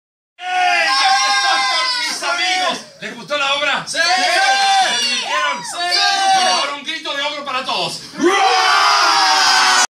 Shrek y sus amigos Violetteros
Los chicos de Violetta ya se sumaron a la ‪#‎Shrekmania‬ ¿Vos cuándo venis a vernos? Shrek - El Musical
Funciones de Jueves a Sábados 19.30hs y Domingos 18.30hs
Podes conseguir tus entradas en el Teatro MAIPO o en Plateanet
Gracias Pablo Sultani por el video.